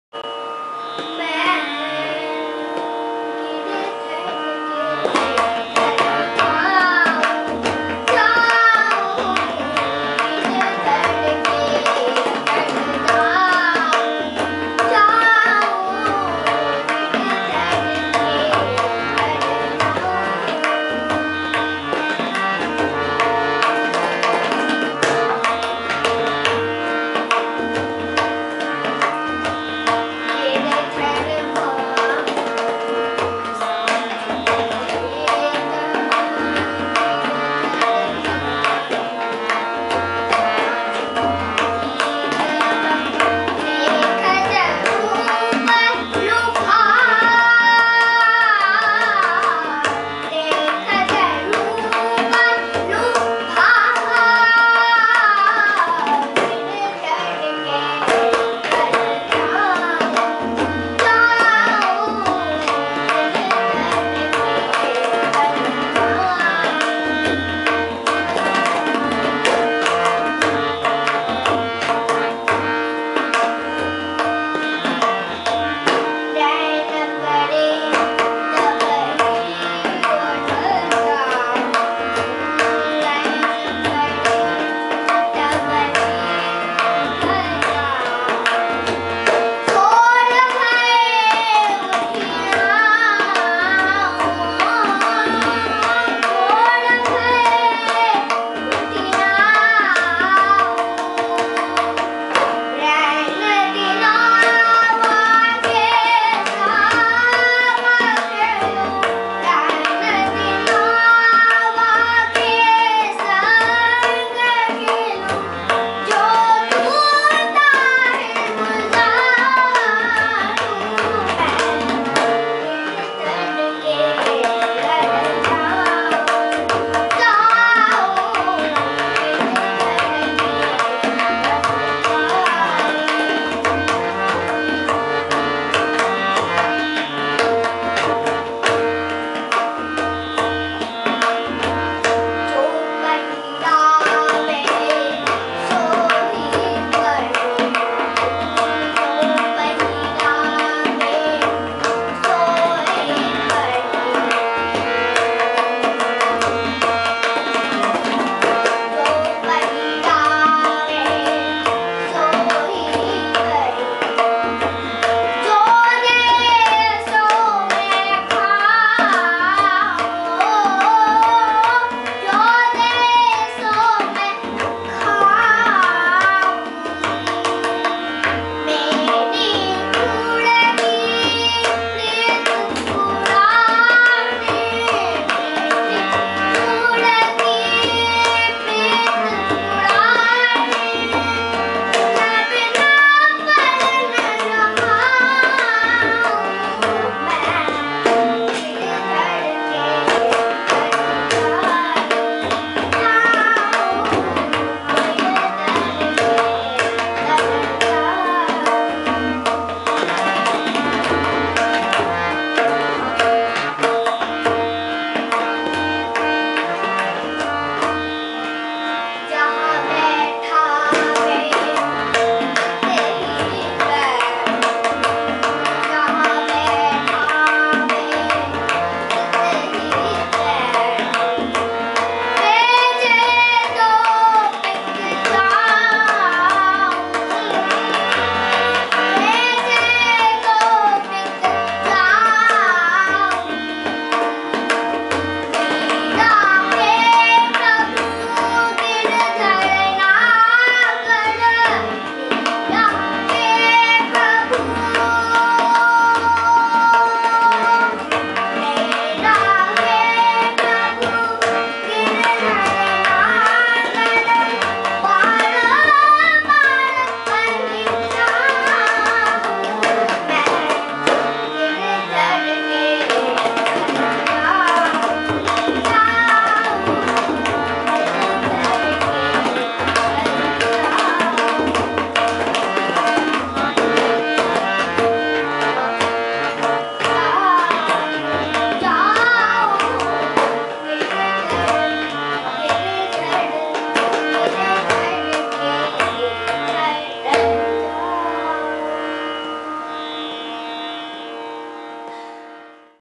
Amazing!! Prodigy Child Jayantika Dey d/o Devashish Dey singing Mirabai Bhajan
Sister of Shubhanker Dey is singing Mirabai Bhajan at the age of 6 years Child prodigy. She is learning at Shilpayan the Music Hub.